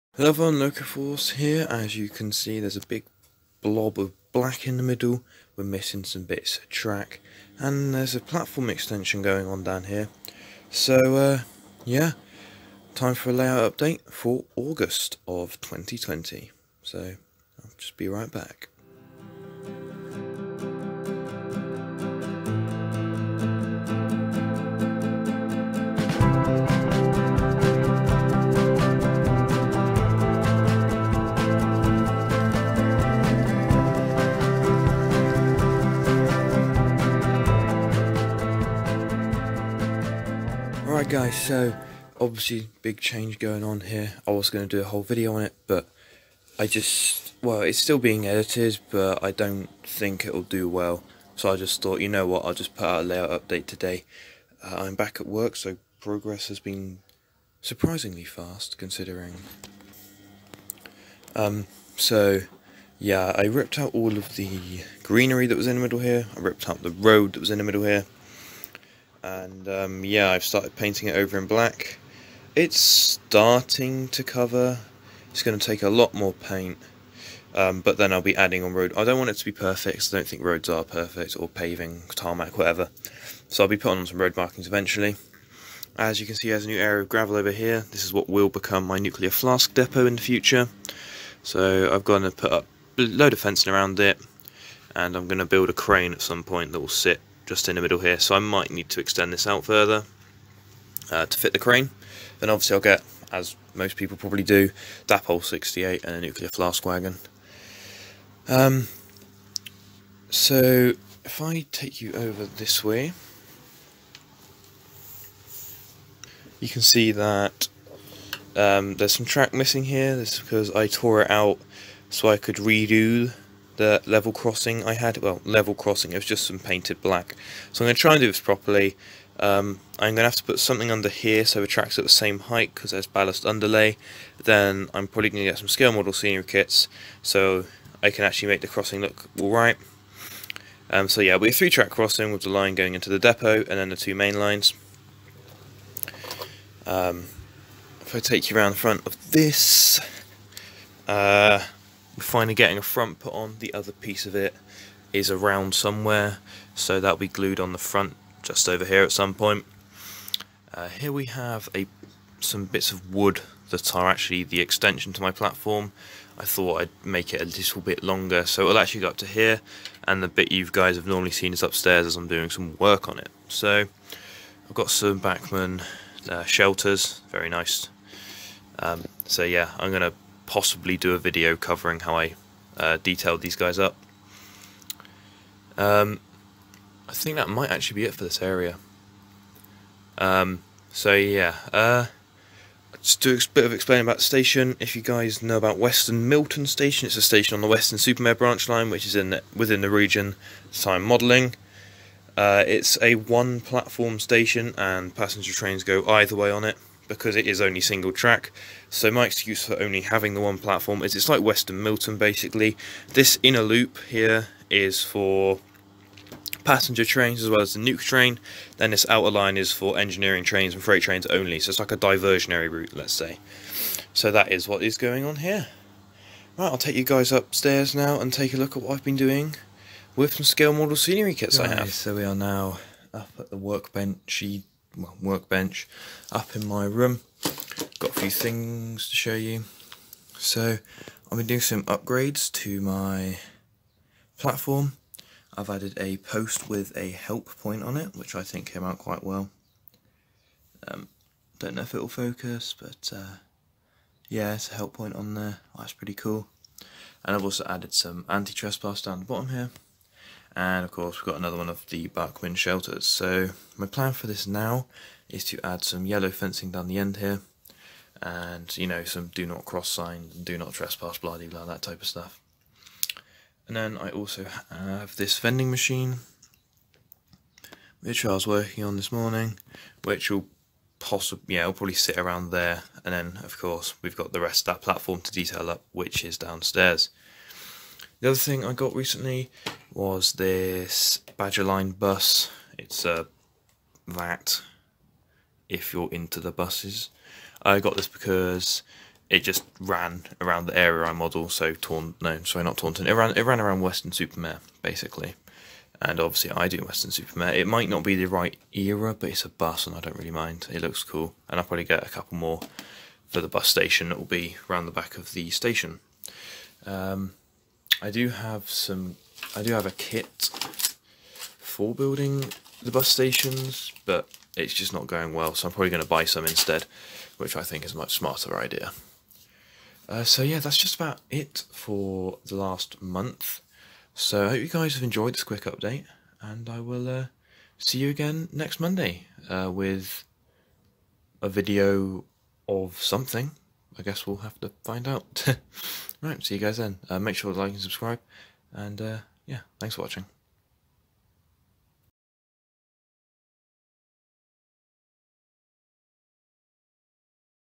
OO GAUGE - St Brians - August Update - MASSIVE Scenic Update
Over the past 2 weeks i have torn up a large area of the layout to make way for new scenery which will take the form of a longer stations based off weston milton on the weston supermare branch line, a car park and a nuclear flask depot. next video will hopefully be me detailing up my platforms, however this is subject to work!

Hope you enjoyed the latest video from Locoforce!